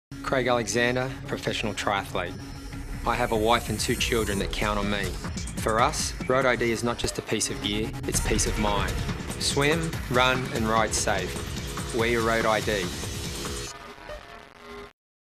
Road ID Craig Alexander :15 HD TV Commercial
Road ID wearer and two-time Ironman World Champion Craig "Crowie" Alexander struts his stuff in his first Road ID TV commercial.